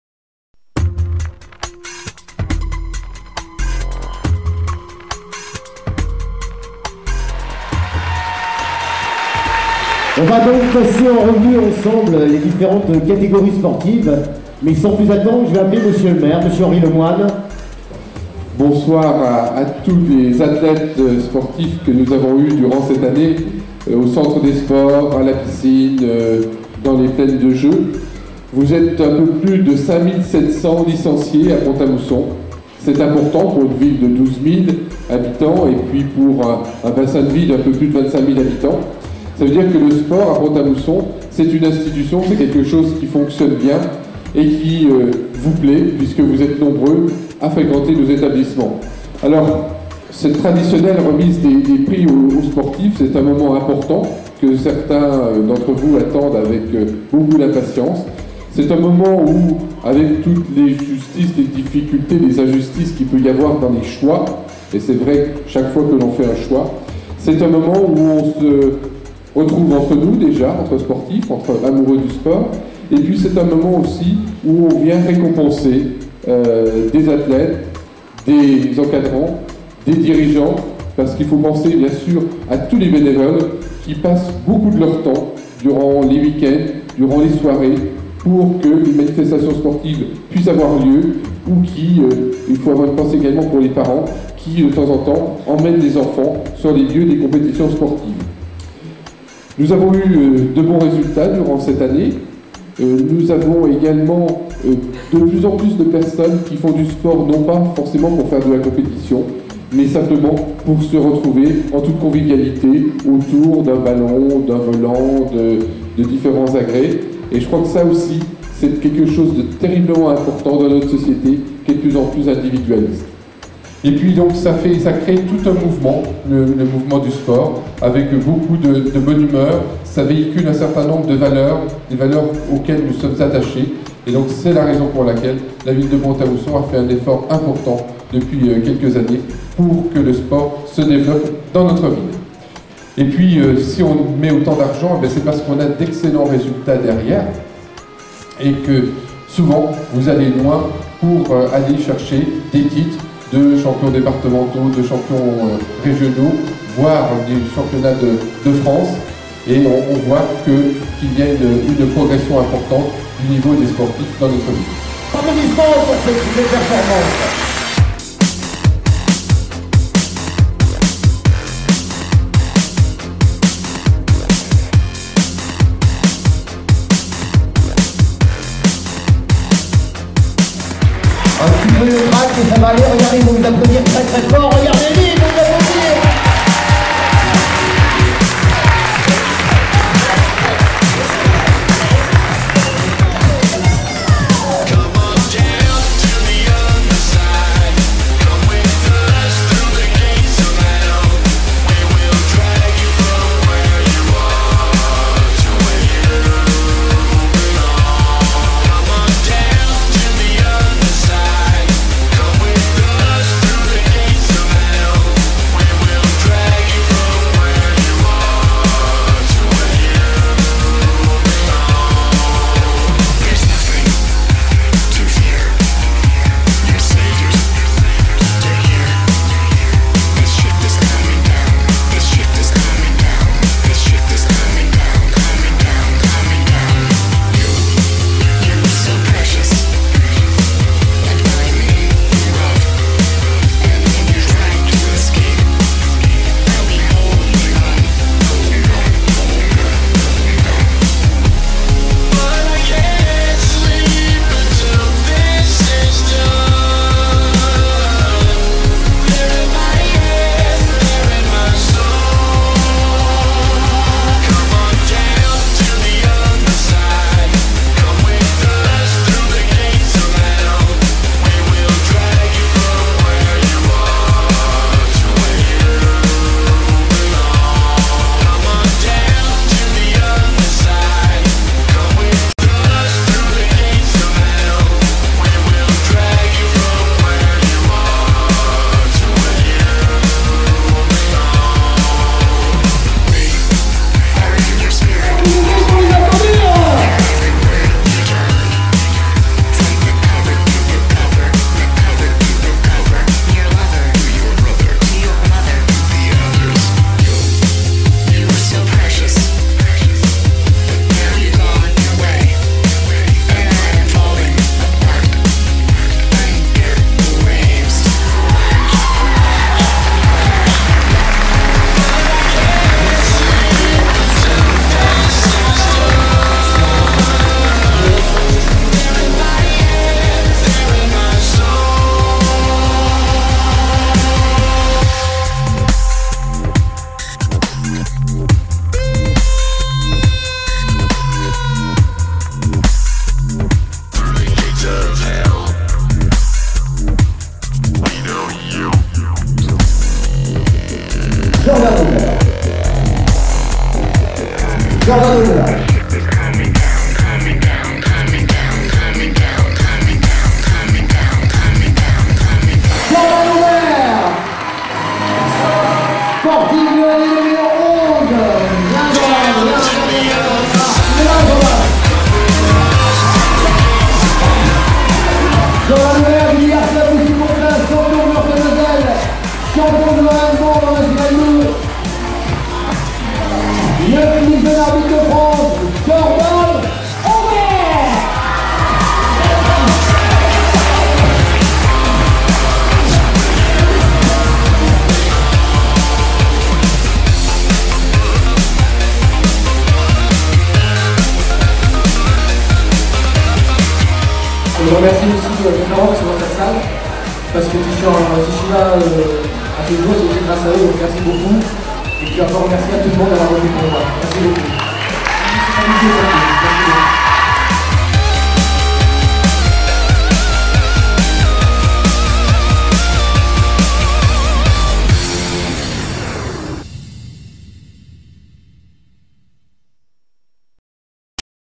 Soirée des sportifs 2011
Soirée de gala à l'Espace Montrichard où plus de 200 sportifs furent récompensés pour leurs performances en 2011. Le sport à Pont-à-Mousson affiche une belle vitalité et mérite plus que jamais son label de "Ville sportive" Jordan Aubert et l'A-S Badminton raflent respectivement les titre de sportif et club de l'année 2011.